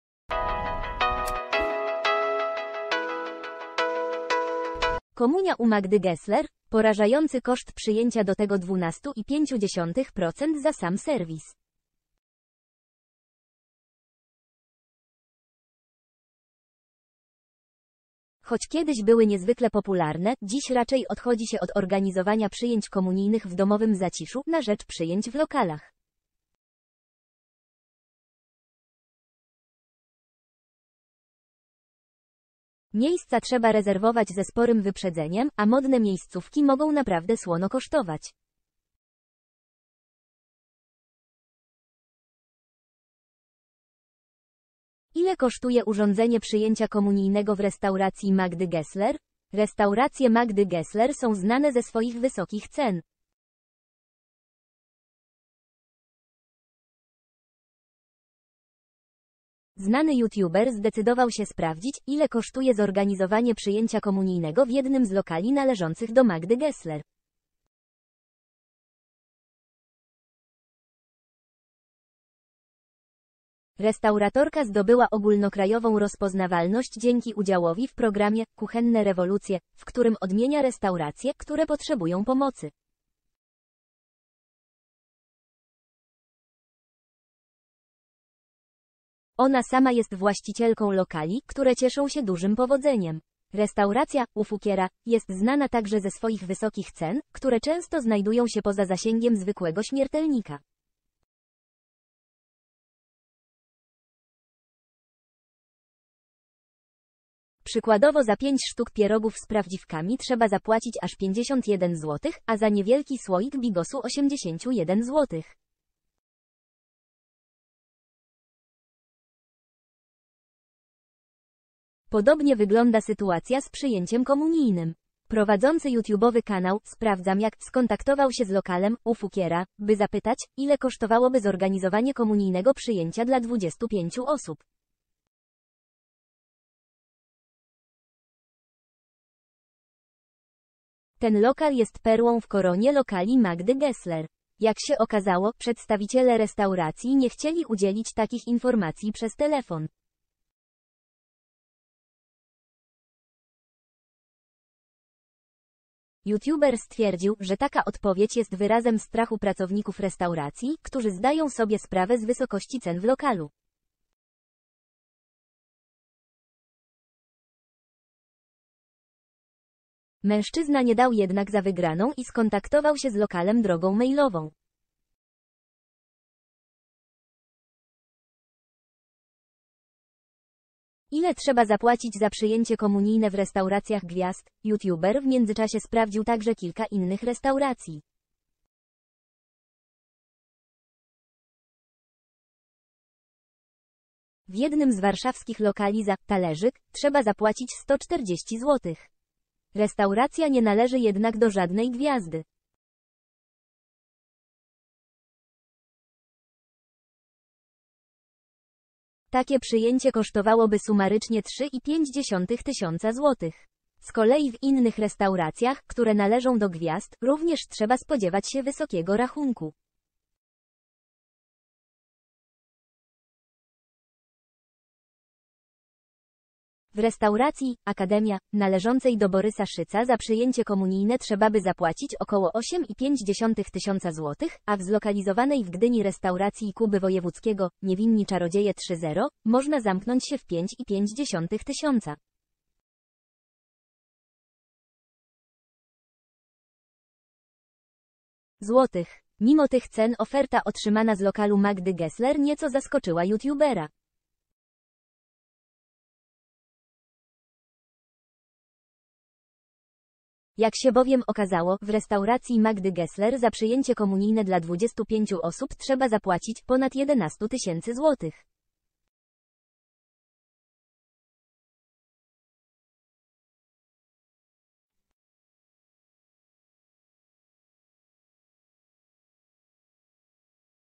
Komunia u Magdy Gessler? Porażający koszt przyjęcia do tego 12,5% za sam serwis
Choć kiedyś były niezwykle popularne, dziś raczej odchodzi się od organizowania przyjęć komunijnych w domowym zaciszu, na rzecz przyjęć w lokalach. Miejsca trzeba rezerwować ze sporym wyprzedzeniem, a modne miejscówki mogą naprawdę słono kosztować. Ile kosztuje urządzenie przyjęcia komunijnego w restauracji Magdy Gessler?
Restauracje Magdy Gessler są znane ze swoich wysokich cen
Znany youtuber zdecydował się spra  ...